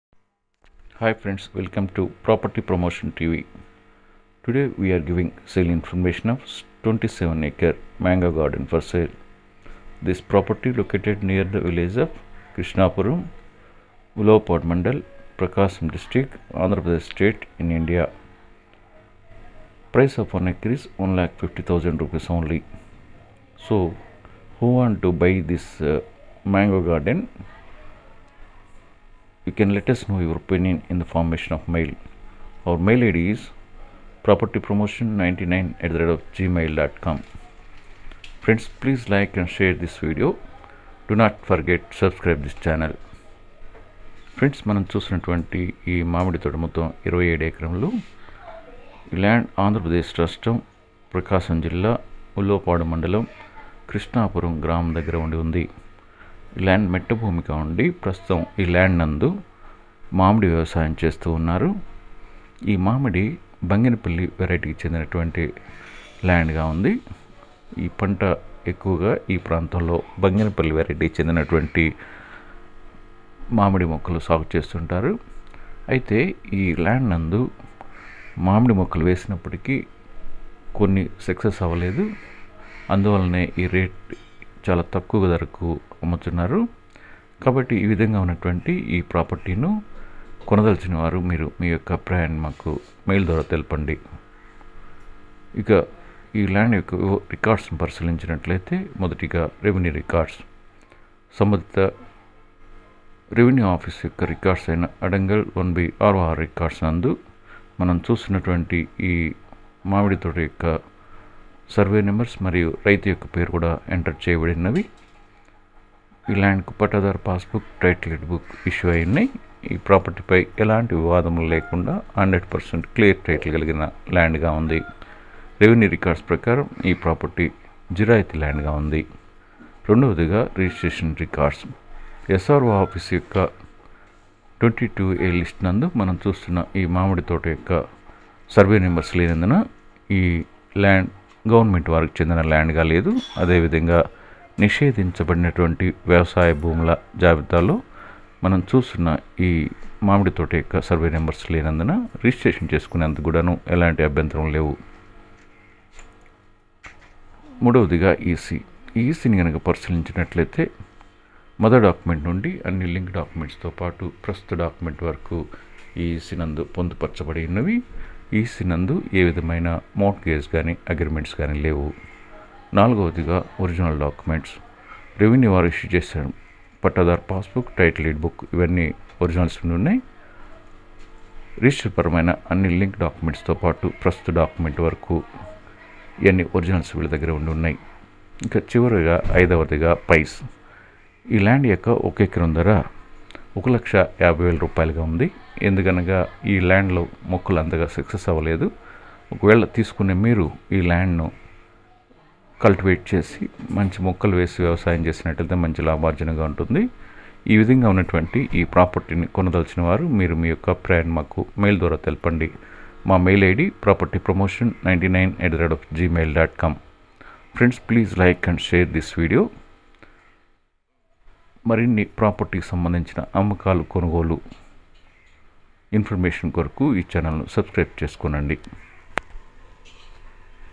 27 ACRE MANGO GARDEN FOR SALE | CHEAP PRICE PROPERTY SALE | COST PER ACRE ₹ 1,50,000 | PROPERTY TV
27 Acre mango Garden For Sale | Cheap Price Property For Sale | Cost Per Acre ₹ 1,50,000 | Property Tv

Hi, friends welcome to PROPERTY PROMOTION TV. Today we are giving sale information of 27 Acres of mango tree cultivation land for sale With Agricultural Property For Sale With Clear title Property.Total 27Acres of clear title Cheap Price Mango Garden Agricultural Property Land.
This land located near the Village of Krishanpuram Village, Ulavapadu Mandal, Prakasam District, Andhra Pradesh State in India. The price of one acre is ₹ 1,50,000
So who wants to buy real estate property, you can let us know your opening in the formation of mail.